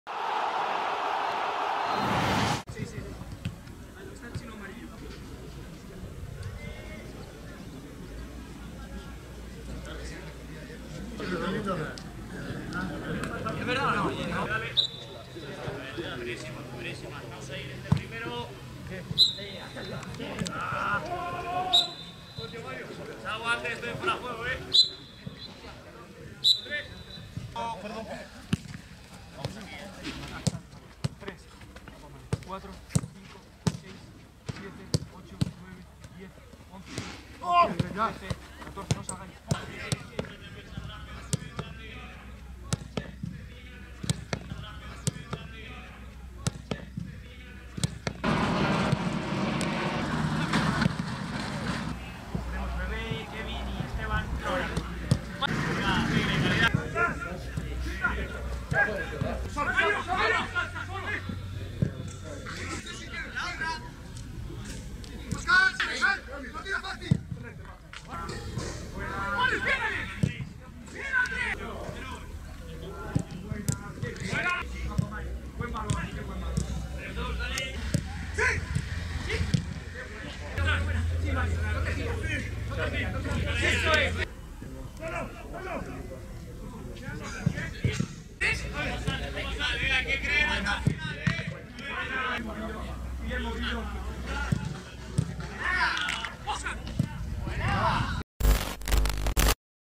Entrenamiento del Rayo Vallecano en Lezama
Sesión de recuperación del primer equipo del Rayo Vallecano, en Lezama, tras el partido ante el Athletic Club en San Mamés.